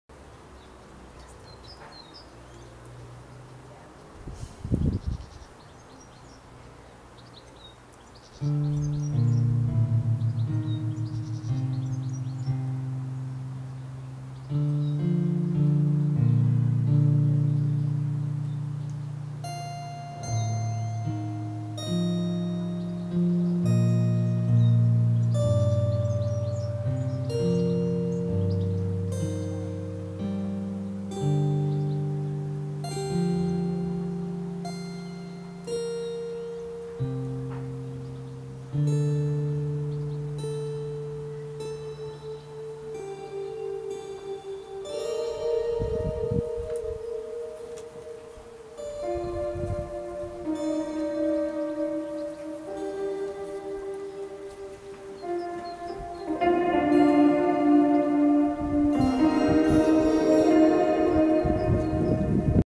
anrimeal - live from the garden - 1
performing with flowers + trees + water + metal cans

thank you, Penny Coutts for helping me film!